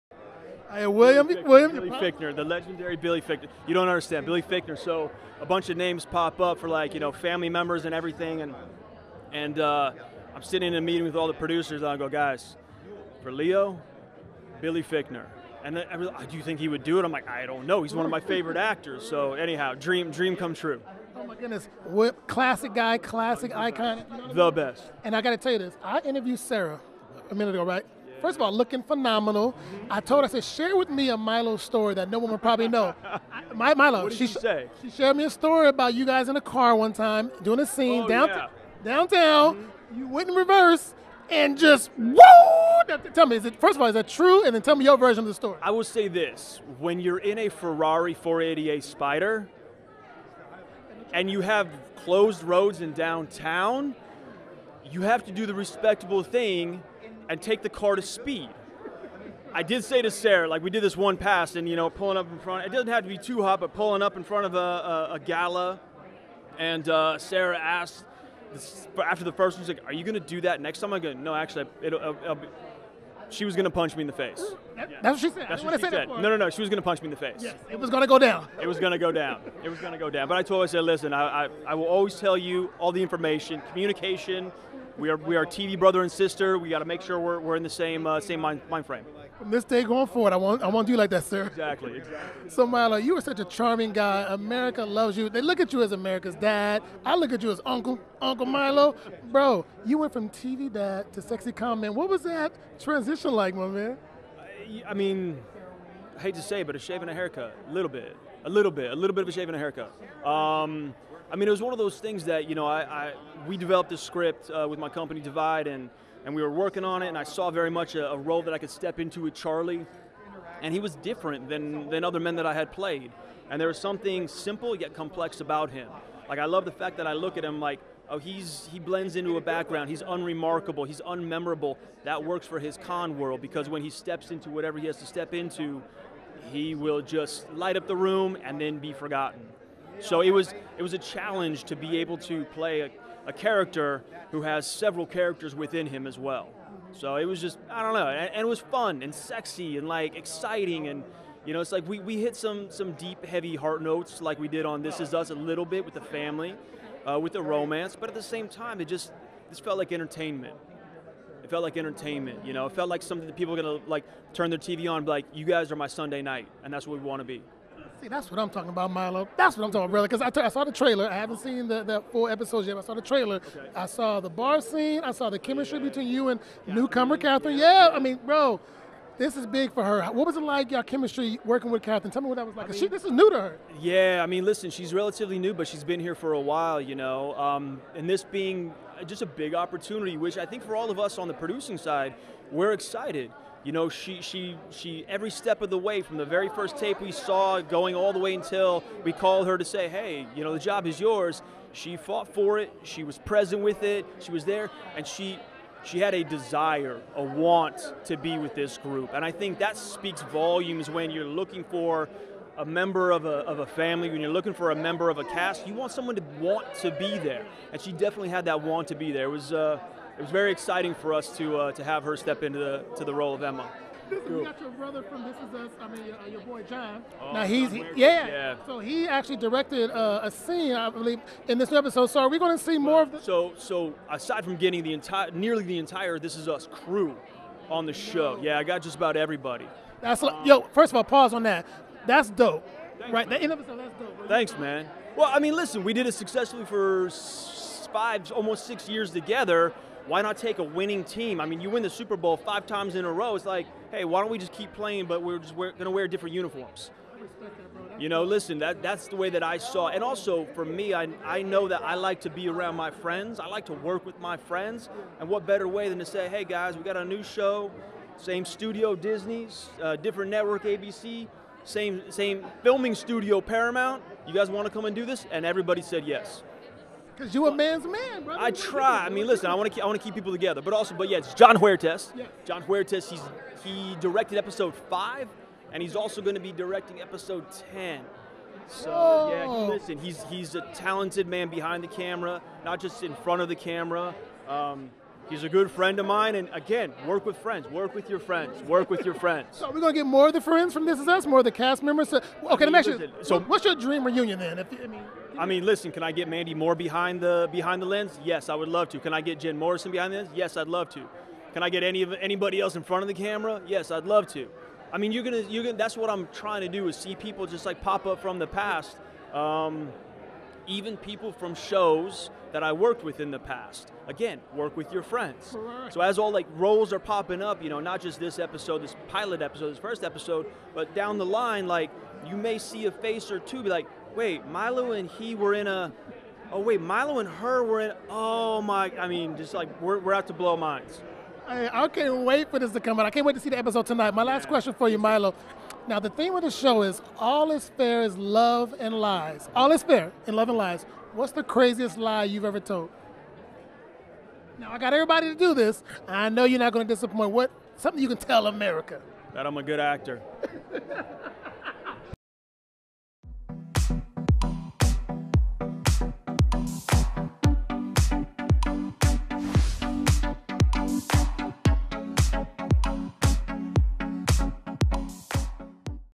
Milo Ventimiglia Wants Mandy Moore To Direct An Ep. Of ‘The Company You Keep’
Milo Ventimiglia spoke with Access Hollywood at the premiere of “The Company You Keep” and he shared about the series. He also says he would love his former “This Is Us” co-star Mandy Moore to direct an episode. “The Company You Keep” premieres Feb. 19 on ABC.